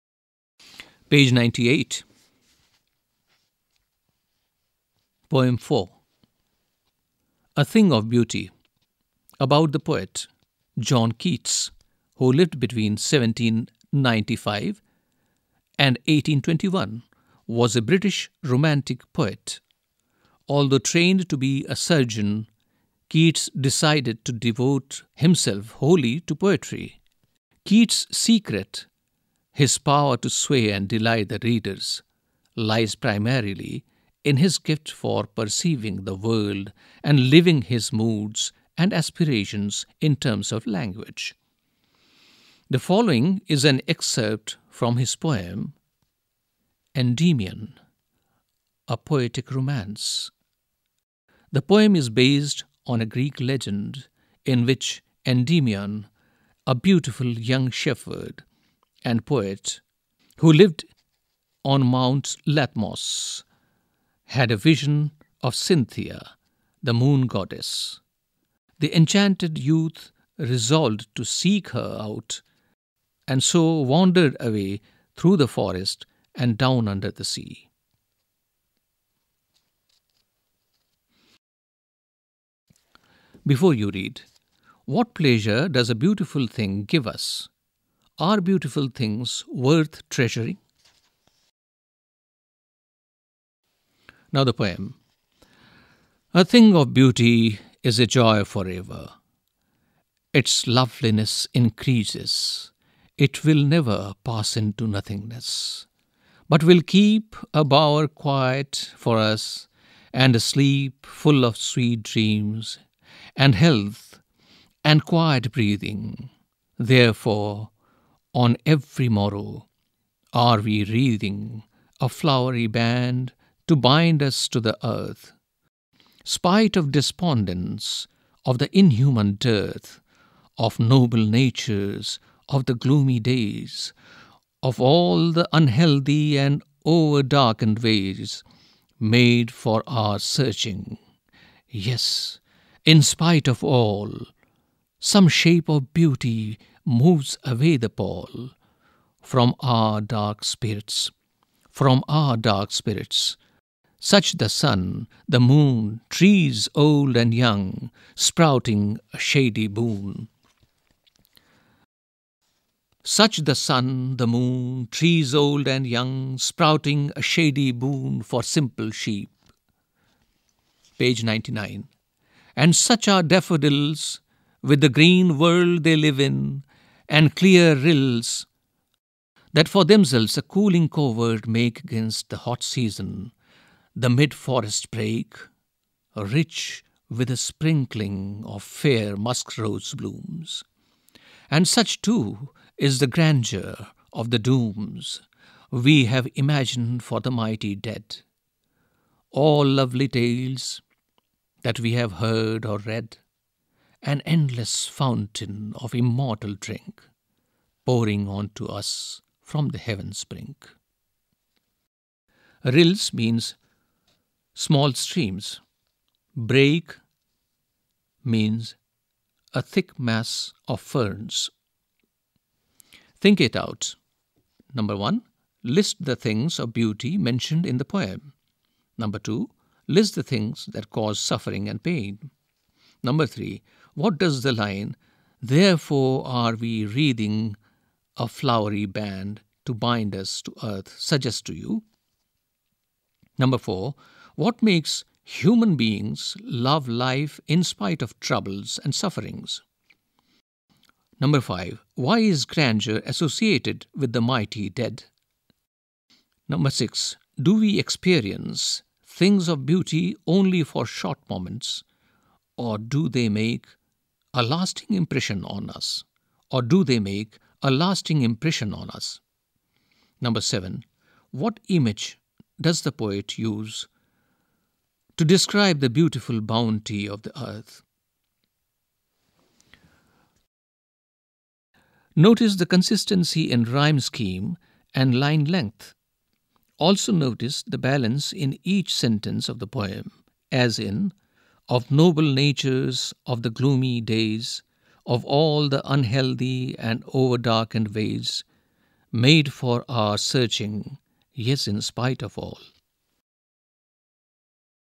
A Thing Of Beauty
A Thing of Beauty is by John Keats. Beautiful things give us pleasure. Find out are the beautiful things worth treasuring?